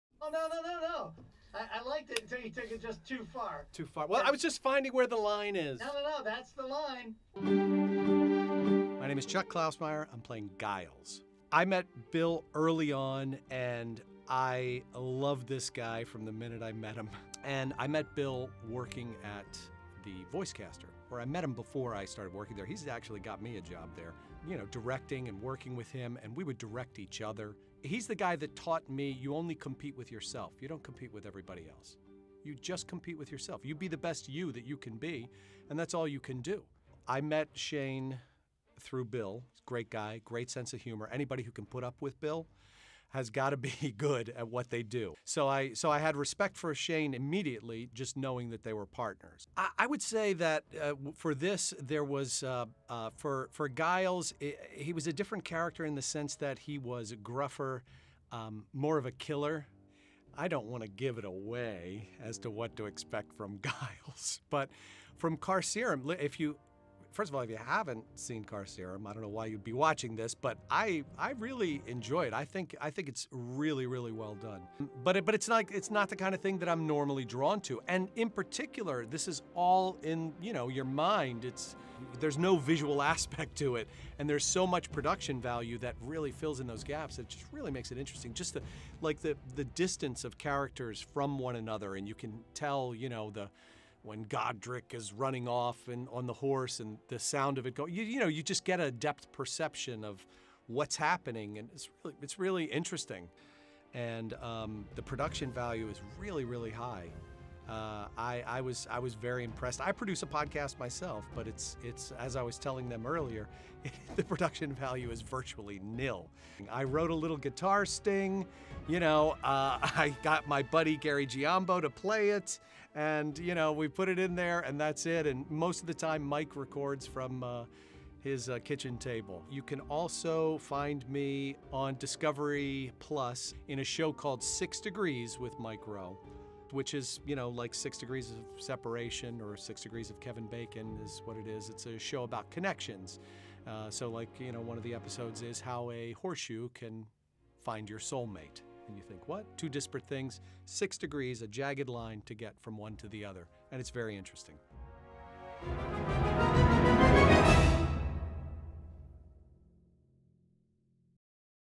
Chuck Klausmeyer - BTS Interview
Chuck Klausmeyer stops by to talk about starring in Carcerem and his own experiences with Audio projects